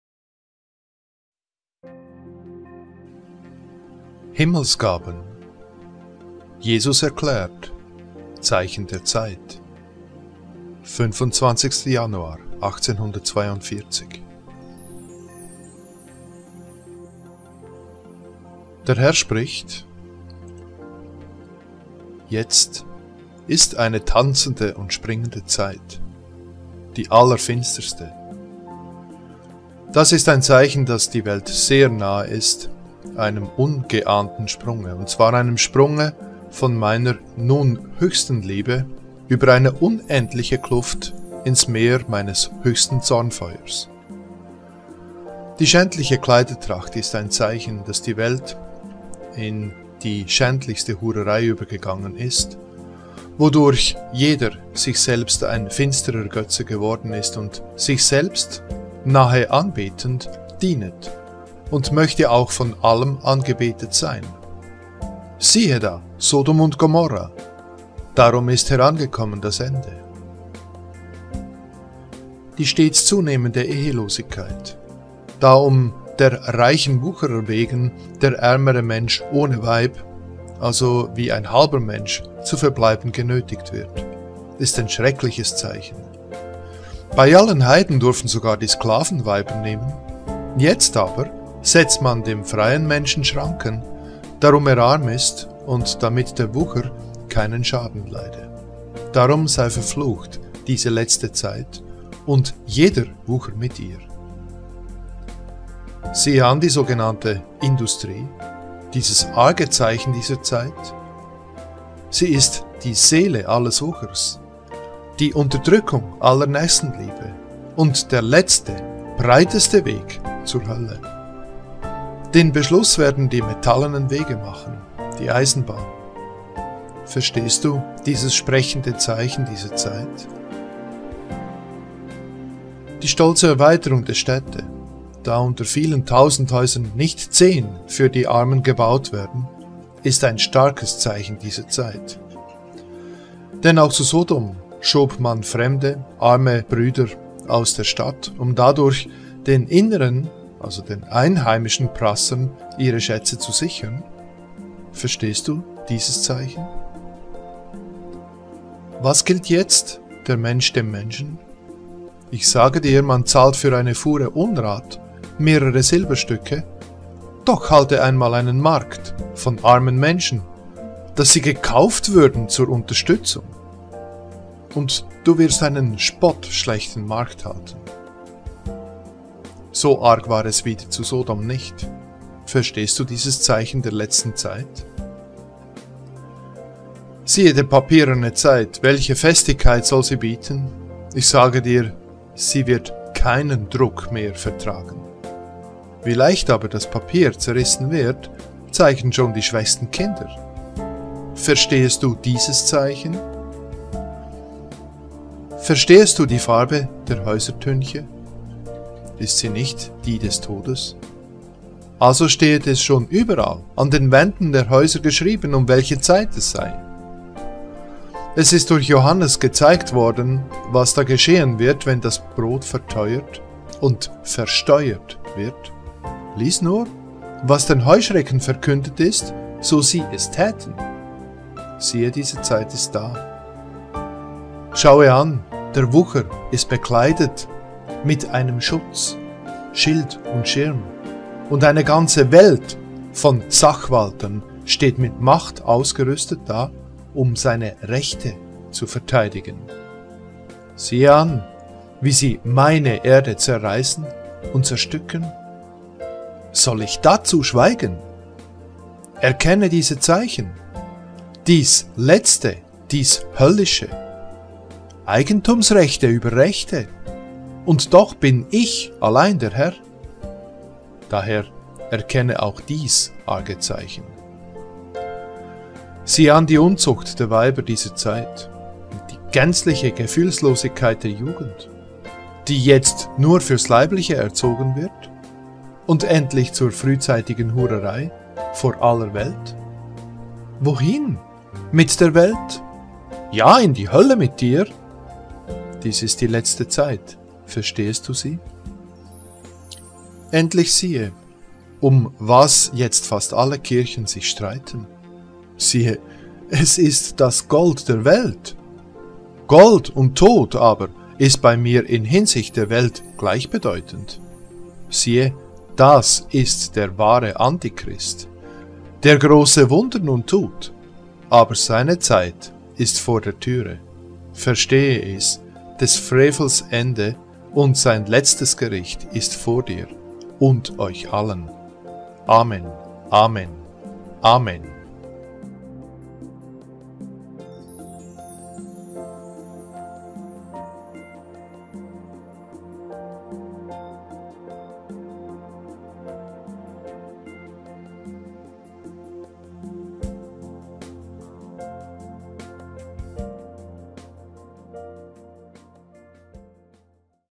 ZEICHEN DER ZEIT WUCHER, SCHÄNDLICHE KLEIDERTRACHT, INDUSTRIE USW ❤️ Himmelsgaben Jakob Lorber 7
Auszug aus der Botschaft !
Zeichen der Zeit… Wucher, schändliche Kleidertracht, Industrie usw
Himmelsgaben – 25. Januar 1842

1. Jetzt ist eine tanzende und springende Zeit, die allerfinsterste! Das ist ein Zeichen, dass die Welt sehr nahe ist einem ungeahnten Sprunge, und zwar einem Sprunge von Meiner nun höchsten Liebe über eine unendliche Kluft ins Meer Meines höchsten Zornfeuers!

2. Die schändliche Kleidertracht ist ein Zeichen, dass die Welt in die schändlichste Hurerei übergegangen ist, wodurch jeder sich selbst ein finsterer Götze geworden ist und sich selbst, nahe anbetend, dienet und möchte auch vor allem angebetet sein. – Siehe da Sodom und Gomorra! – Darum ist herangekommen das Ende!

3. Die stets zunehmende Ehelosigkeit, da um der reichen Wucherer wegen der ärmere Mensch ohne Weib, also wie ein halber Mensch, zu verbleiben genötigt wird, ist ein schreckliches Zeichen! Bei allen Heiden durften sogar die Sklaven Weiber nehmen. Jetzt aber setzt man dem freien Menschen Schranken, darum er arm ist und damit der Wucher keinen Schaden leide! – Darum sei verflucht diese letzte Zeit und jeder Wucher mit ihr!

4. Siehe an die sogenannte Industrie, dieses arge Zeichen dieser Zeit! Sie ist die Seele alles Wuchers, die Unterdrückung aller Nächstenliebe und der letzte, breiteste Weg zur Hölle. Den Beschluss werden die metallenen Wege machen (Eisenbahn)! – Verstehst du dieses sprechende Zeichen dieser Zeit!?

5. Die stolze Erweiterung der Städte, da unter vielen tausend Häusern nicht zehn für die Armen gebaut werden, ist ein starkes Zeichen dieser Zeit. Denn auch zu Sodom schob man fremde arme Brüder aus der Stadt, um dadurch den inneren (einheimischen) Prassern ihre Schätze zu sichern? – Verstehst du diese Zeichen?

6. Was gilt jetzt der Mensch dem Menschen? – Ich sage dir, man zahlt für eine Fuhre Unrat mehrere Silberstücke; doch halte einmal einen Markt von armen Menschen, dass sie gekauft würden zur Unterstützung, und du wirst einen spottschlechten Markt halten! So arg war es wieder zu Sodom nicht! – Verstehest du dieses Zeichen der letzten Zeit?

7. Siehe die papierne Zeit! – Welche Festigkeit soll sie bieten? Ich sage dir, sie wird keinen Druck mehr vertragen! Wie leicht aber das Papier zerrissen wird, zeigen schon die schwächsten Kinder! – Verstehest du dieses Zeichen?

8. Verstehest du die Farbe der Häusertünche? – Ist sie nicht die des Todes? – Also stehet es schon überall an den Wänden der Häuser geschrieben, um welche Zeit es sei!

9. Es ist durch Johannes gezeigt worden, was da geschehen wird, wenn das Brot verteuert und versteuert wird! Lies nur, was den Heuschrecken verkündet ist, so sie es täten! – Siehe, diese Zeit ist da!

10. Schaue an, der Wucher ist bekleidet mit allem Schutz, Schild und Schirme, und eine ganze Welt voll Sachwaltern steht mit Macht ausgerüstet da, um seine „Rechte“ zu verteidigen. Siehe an, wie sie Meine Erde zerreissen und zerstücken! Soll Ich dazu schweigen? Erkenne dies Zeichen, dies letzte, dies höllische! Eigentums-Rechte über Rechte! Und doch bin Ich allein der Herr! – Daher erkenne auch dies arge Zeichen!

11. Siehe an die Unzucht der Weiber dieser Zeit und die gänzliche Gefühllosigkeit der Jugend, die jetzt nur fürs Leibliche erzogen wird und endlich zur frühzeitigen Hurerei vor aller Welt! – Wohin mit der Welt?! – Ja, in die Hölle mit ihr! Dies ist die letzte Zeit! – Verstehest du sie?

12. Endlich siehe, um was jetzt fast gar alle Kirchen sich streiten! Siehe, es ist das Gold der Welt. Gold und Tod aber ist bei Mir in Hinsicht der Welt gleichbedeutend! Siehe, das ist der wahre Antichrist, der grosse Wunder nun tut! Aber seine Zeit ist vor der Türe! Verstehe es, des Frevels Ende und sein letztes Gericht ist vor dir und euch allen! Amen. Amen. Amen.

Übersicht der letzten 20 Botschaften !

PERSÖNLICHES WORT VON DEM HERRN!